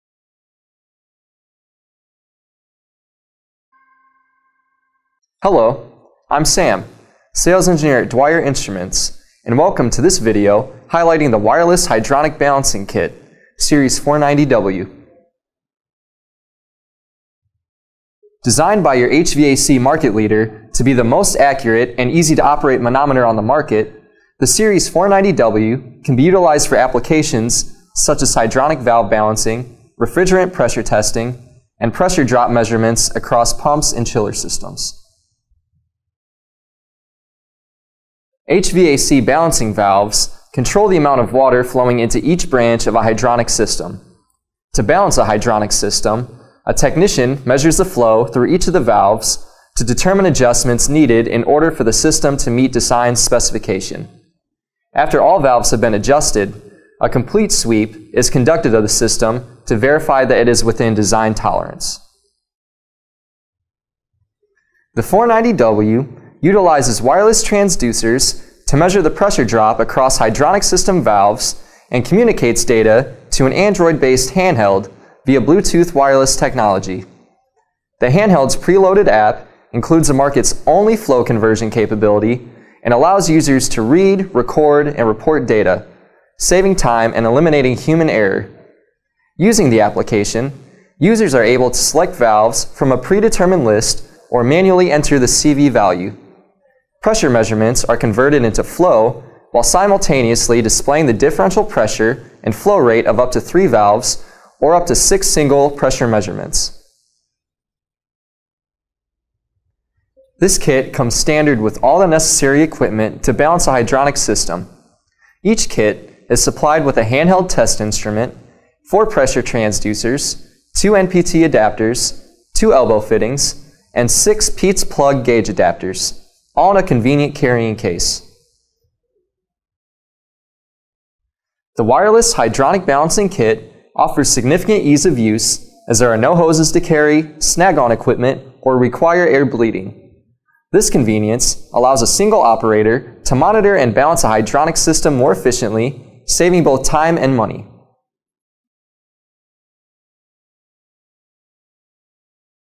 Dwyer Wireless Hydronic Balancing Kit: Overview
In this video, the view will receive an overview of this wireless hydronic balancing kit. Additionally, it will go over the applications, how it works, features, and the included accessories.